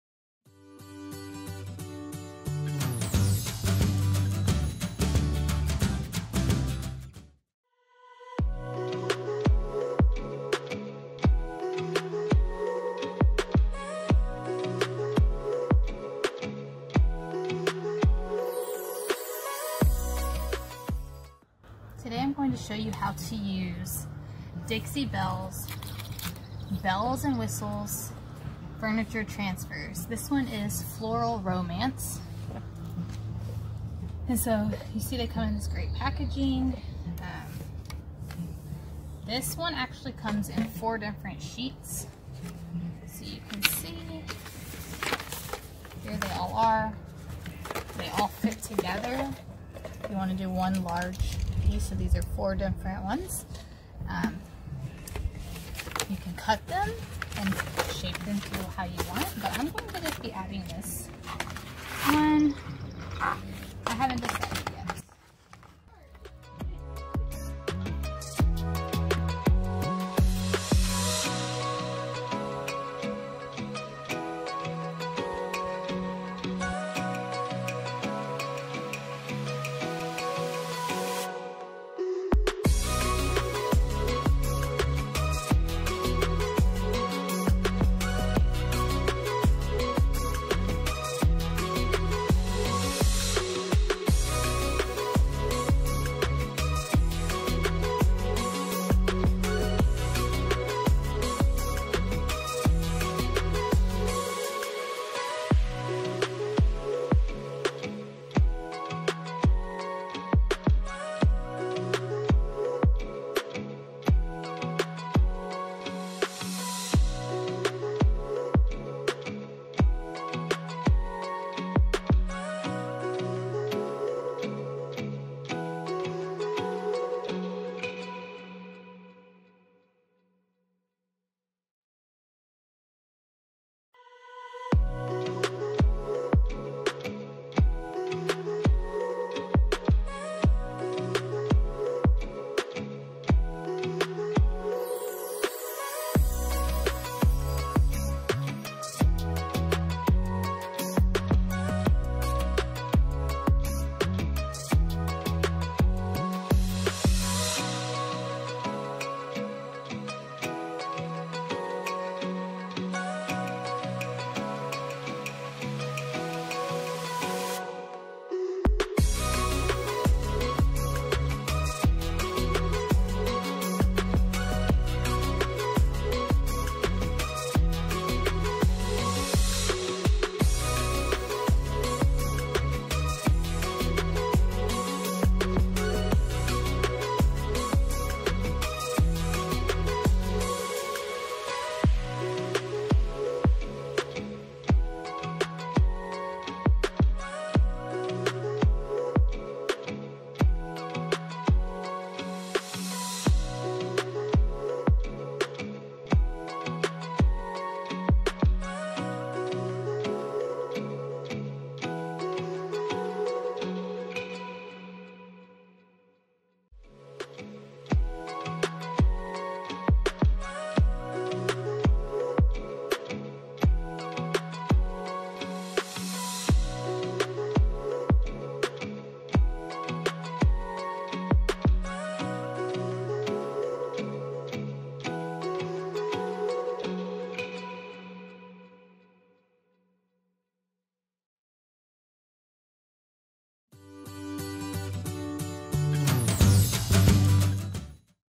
Free Demo: See How Belles and Whistles Rub On Furniture Transfers Work
In this free demo by Breath of Life, see how Belles and Whistles Rub On Furniture Transfers work. You'll get a demonstration of the process and see how easy it is to apply Belles and Whistles rub on transfers to any piece of furniture.

If you're looking to refinish or restore furniture, then be sure to check out this free demo! You'll see how easy it is to apply Belles and Whistles rub on transfers to any piece of furniture, transforming it into a beautiful masterpiece. Don't miss out on this opportunity to see how Belles and Whistles Rub On Furniture Transfers work for yourself!